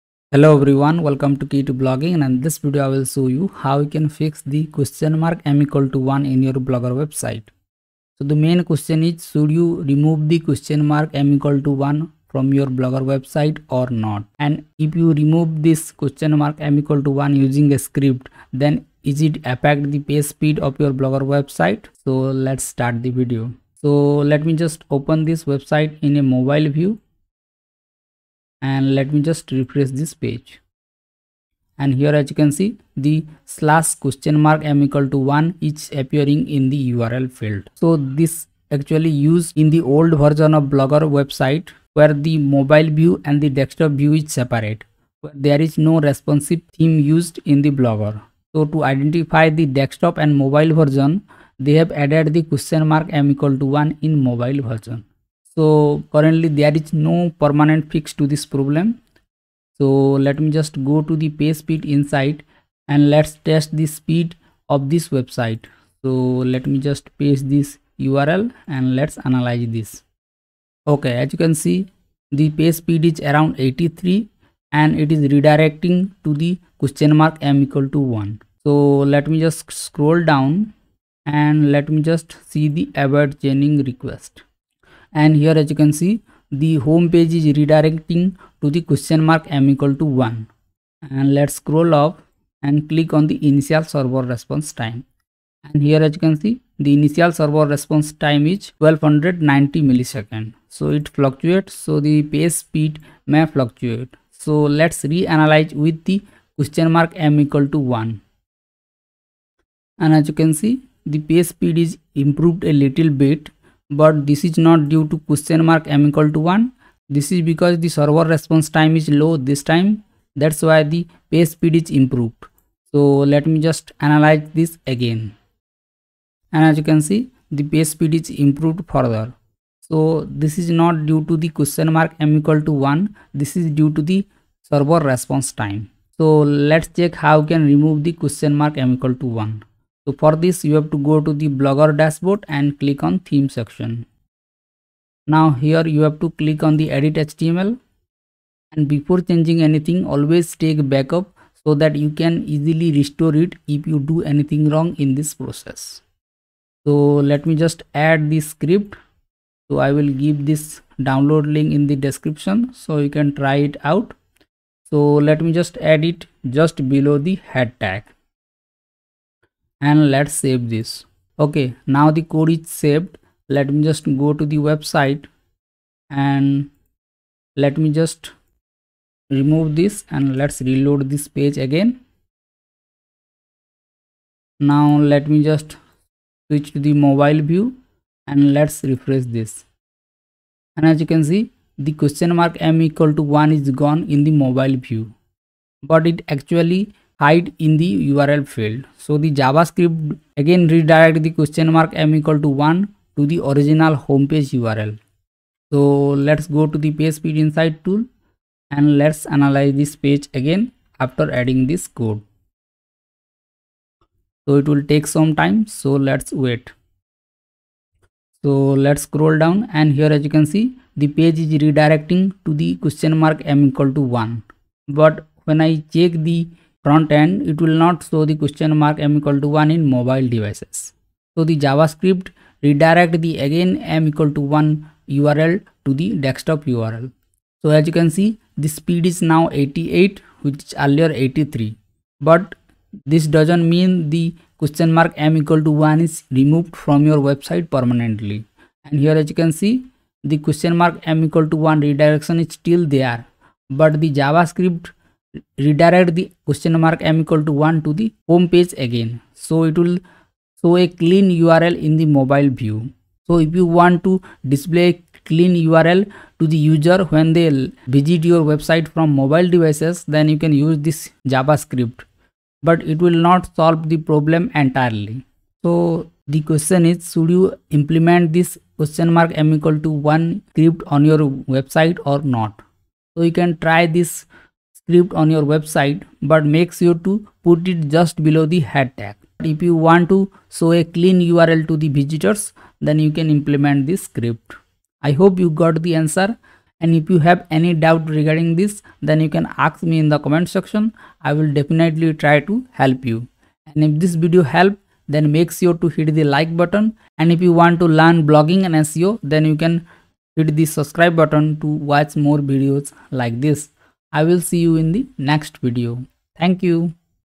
Should you remove ?m=1 from blogger URL | ( Know the Real Truth )
In this video, I will tell you how you can remove ?m=1 from your blogger URL and I will also tell should you remove this or not.

Is removing ?m=1 from Blogger URL affect the SEO or Improve page speed? Well, watch this video till the end to know the answer.

You can delete the ?m=1 from Blogger URL using the above script. Just add the script just below the head tag. The script will hide this ?m=1 from the URL and display a clean URL to the visitors that are accessing the website from Mobile devices.

But, it will not affect the speed performance that much. But, it solves the avoid chain critical request error in page speed insight only when you paste just below the head tag.

It also helps solve canonical issues if any in the Google search console. So, you can try this script on your website but not mandatory.

In fact, I am not using this script on my Blogger website. I hope you got the answer.